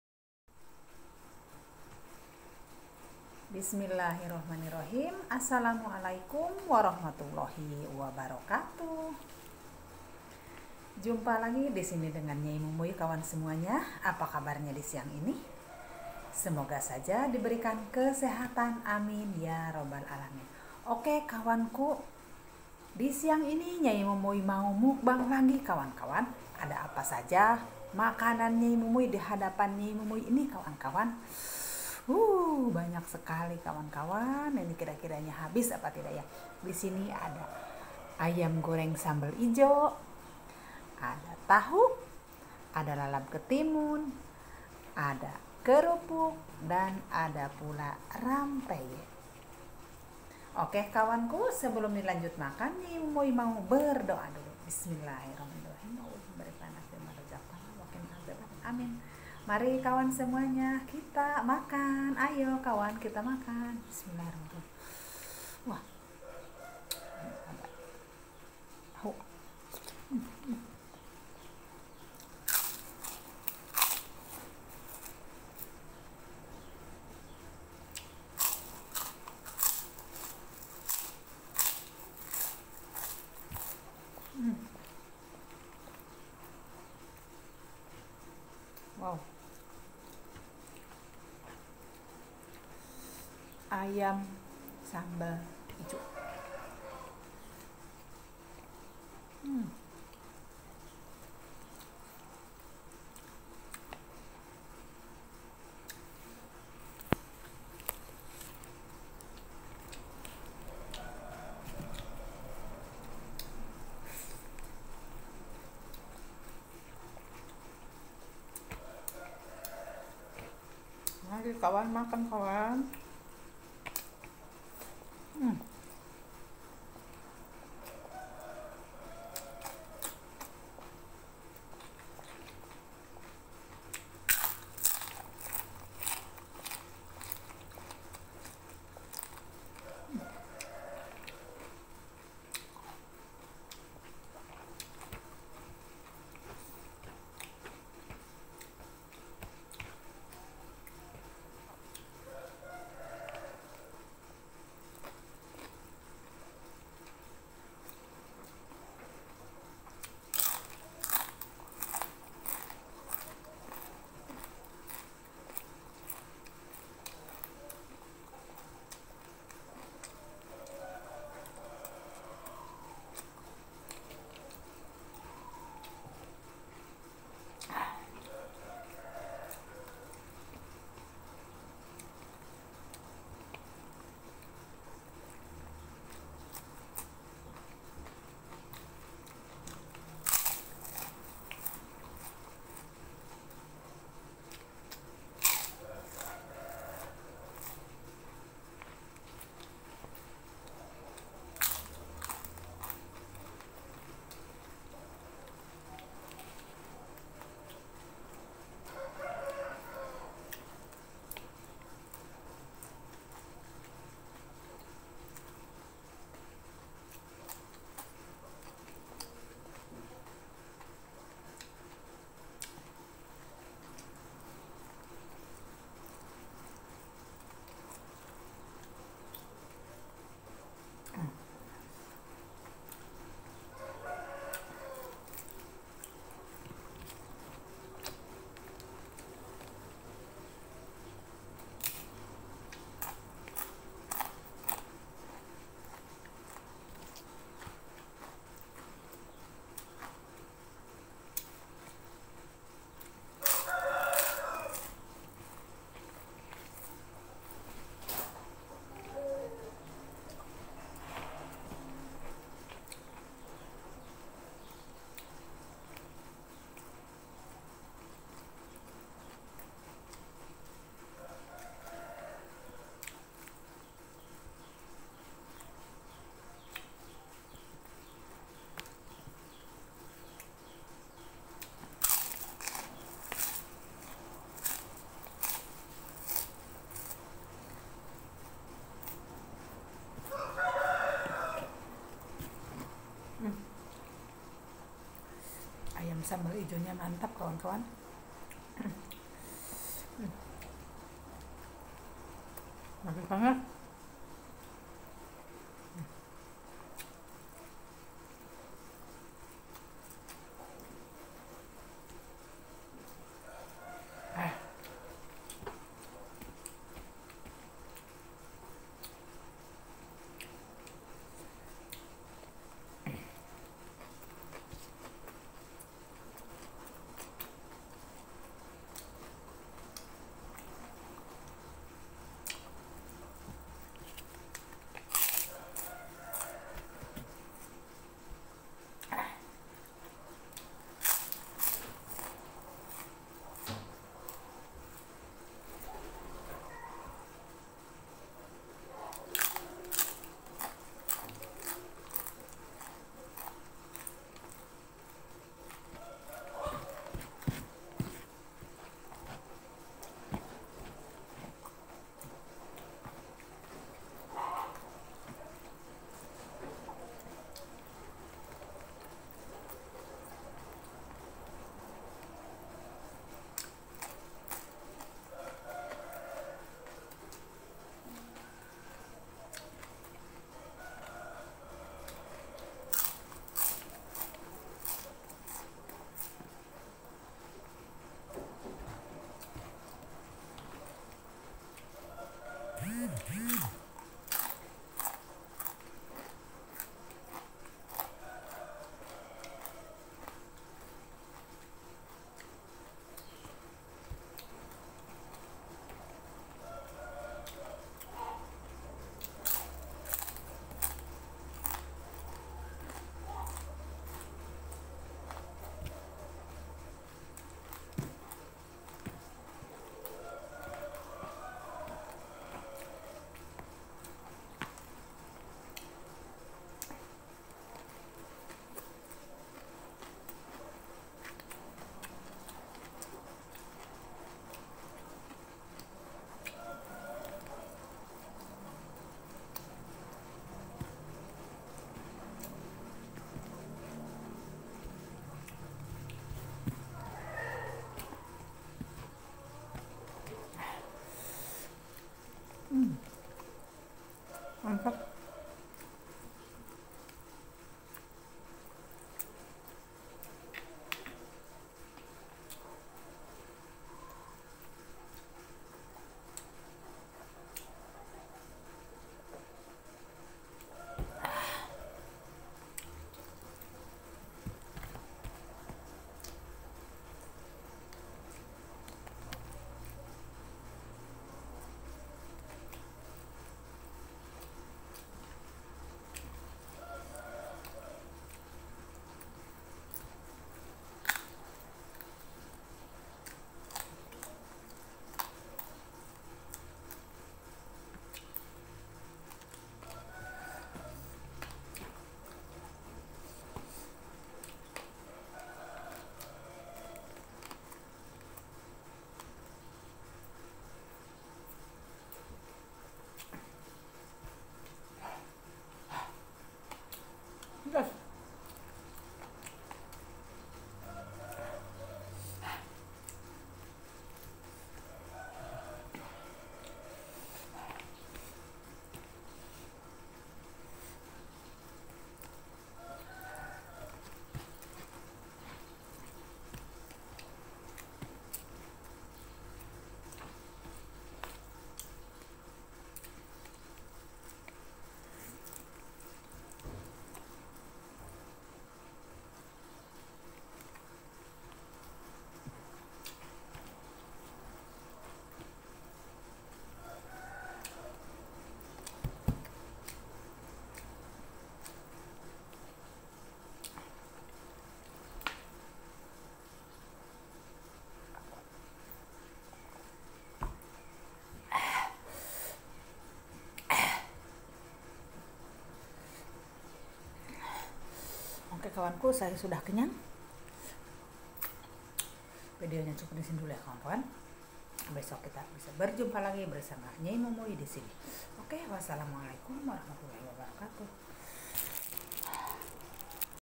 MUKBANG AYAM GORENG SAMBAL IJO..!!TAHU,KERUPUK,RAMPEYEK,KETIMUN..
aslmu alikum kawan semua..di siang ini saya mau mukbang ayam goreng sambal ijo,tahu ,kerupuk,rampeyekdan ketimun ..